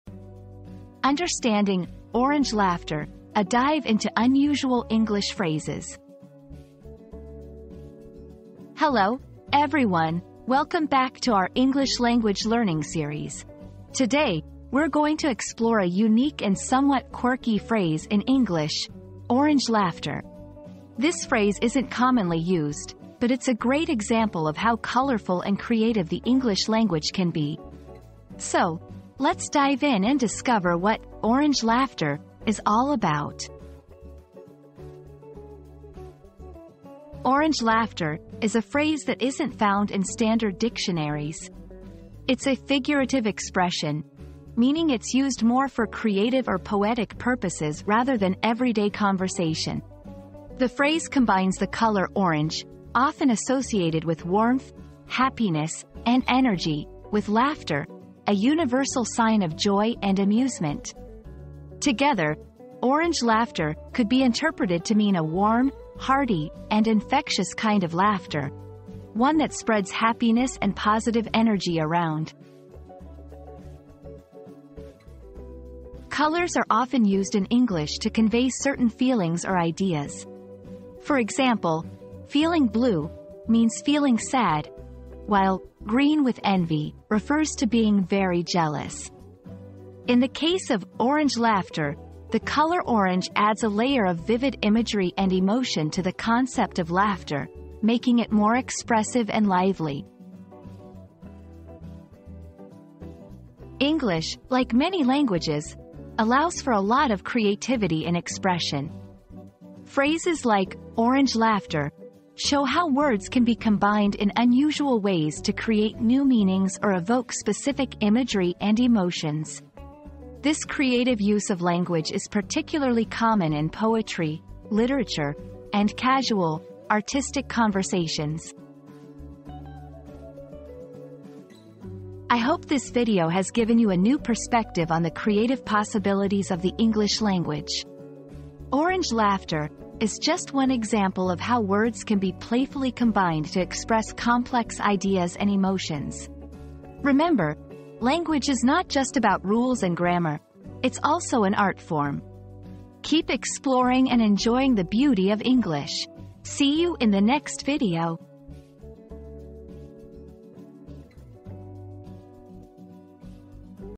Understanding "Orange Laughter": A Dive into Unusual English Phrases
Unraveling the Mystery of 'Orange Laughter': Exploring Unusual English Phrases • Join us on an intriguing journey as we delve into the enigmatic world of 'Orange Laughter' and other uncommon English phrases. Discover the origins, meanings, and cultural significance behind these linguistic curiosities that will leave you both fascinated and entertained.

00:00 • Introduction - Understanding "Orange Laughter": A Dive into Unusual English Phrases
00:36 • What Does "Orange Laughter" Mean?
01:15 • The Use of Color in English Phrases
01:43 • Creative Language in English